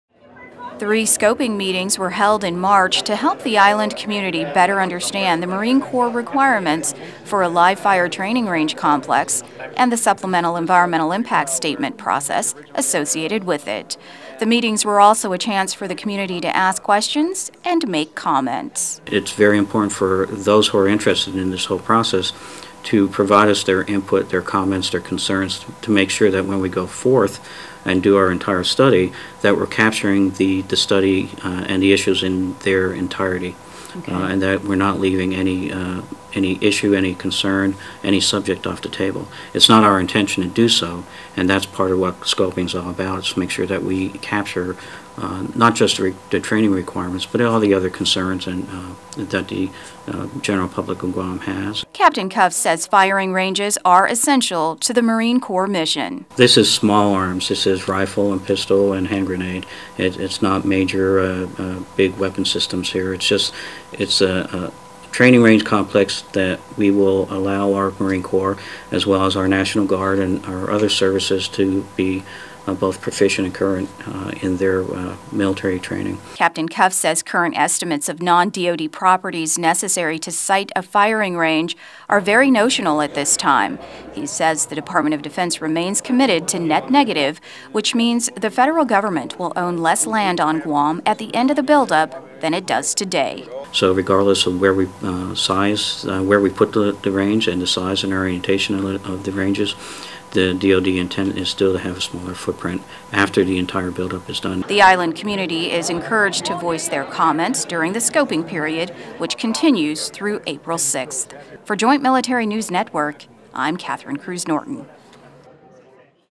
SEIS SCOPING MEETING
Three scoping meetings were held in March, highlighting the need for a live-firing range and the supplemental environmental impact statement process associated with it.